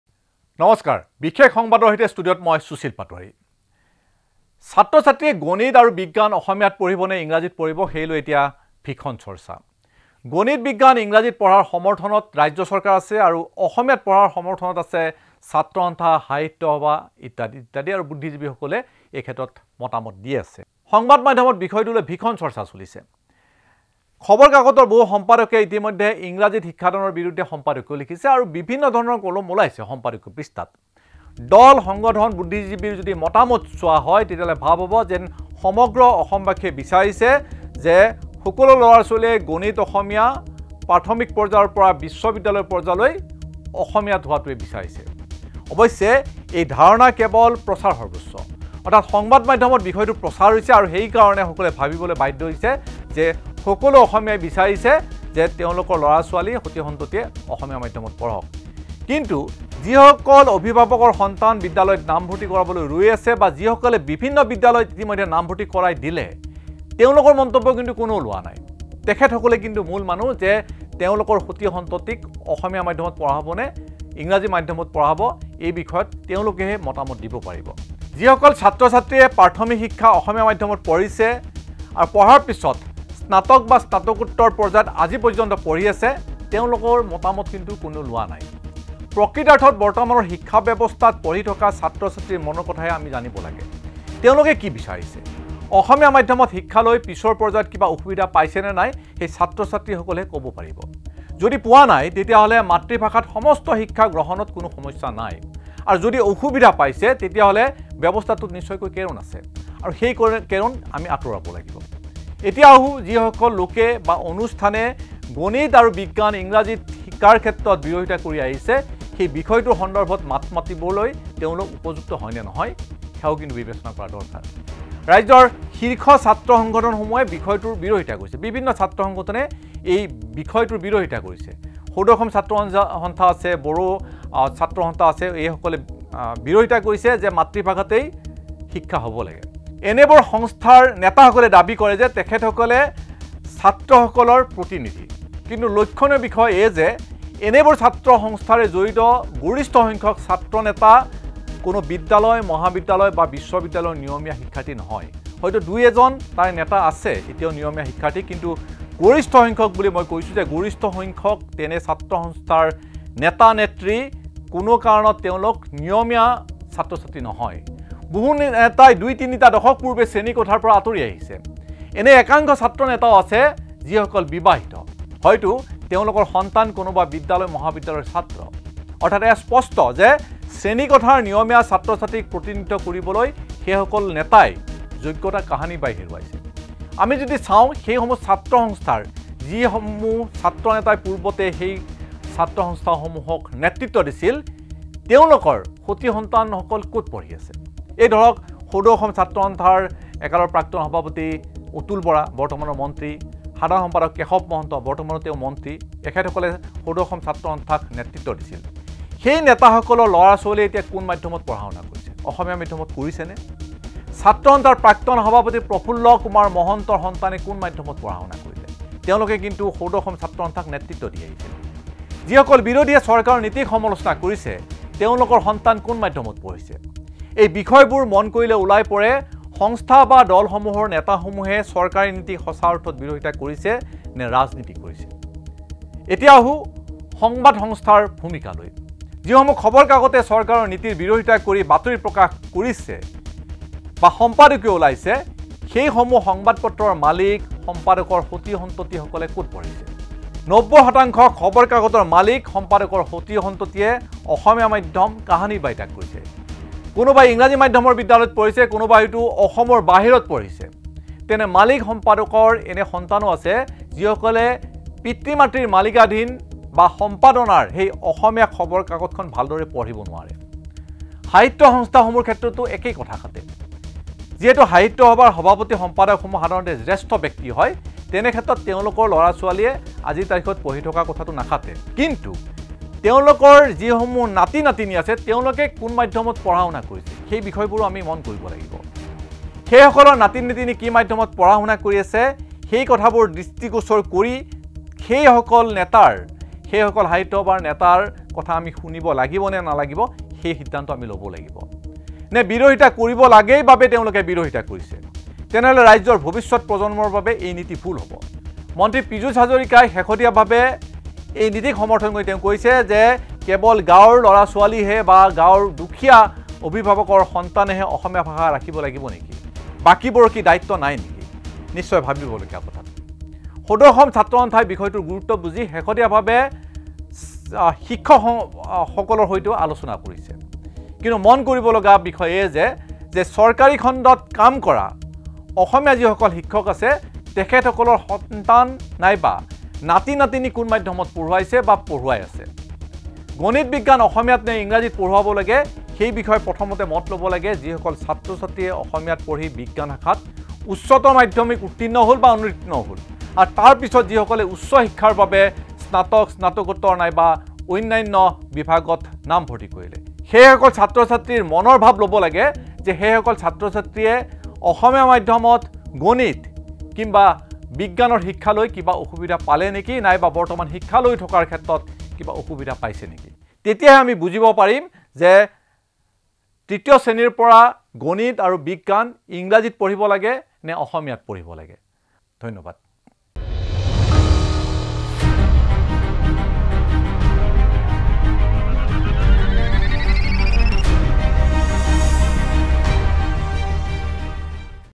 in which medium does newspaper's owner-son study || BISESH SANGBAD || SUSHIL PATOWARY
বাতৰি কাকতৰ মালিক-সন্তানে পঢ়ে কোন মাধ্যমত?
গণিত, বিজ্ঞান, অসমীয়া নে ইংৰাজীত পঢ়িব সেই সিদ্ধান্ত গ্ৰহণ কৰাৰ অধিকাৰ কাৰ হাতত?
বিশেষ সংবাদ || BISESH SANGBAD || SUSHIL PATOWARY ||